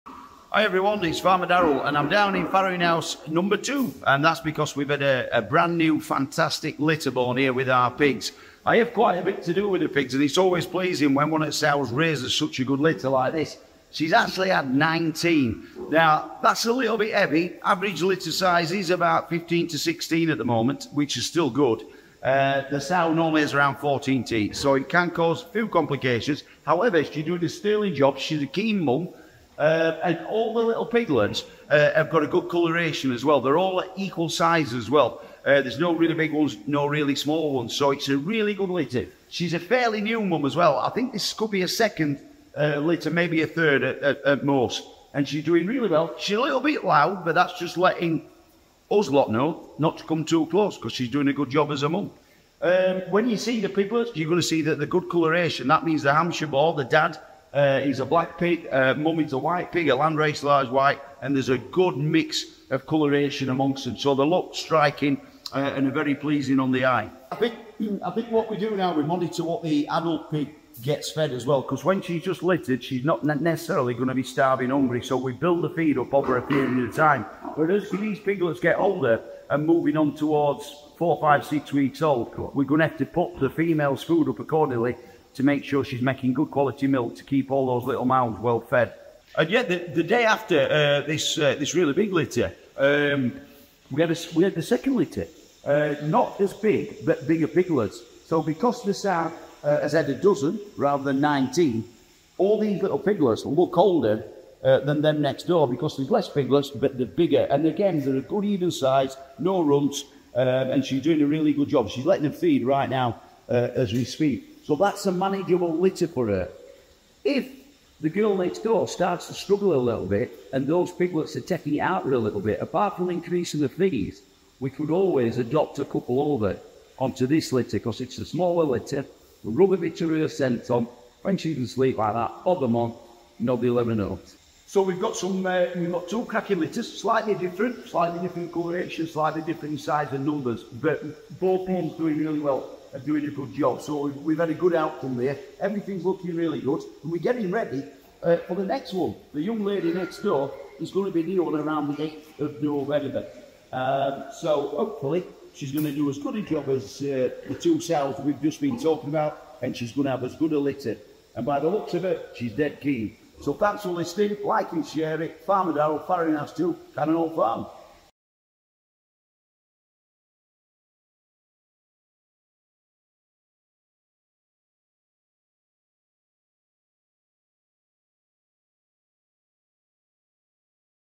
A new litter of pigs - Cannon Hall Farm
Farmer Darrell is down in our farrowing houses where one of our sows has had a whopping 19 piglets.
She's a keen mum - but will she be able to feed them all?